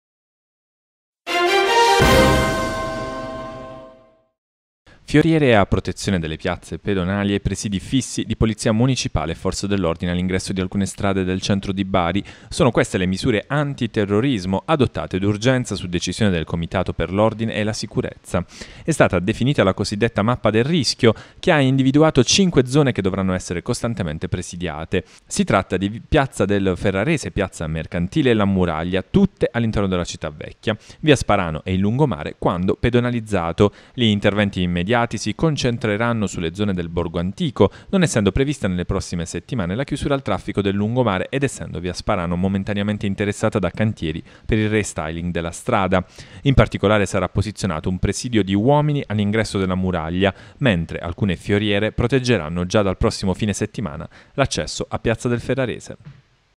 BARI | In 5 piazze di Bari fioriere anti-tir
Le Forze di polizia presidieranno da subito piazze città vecchia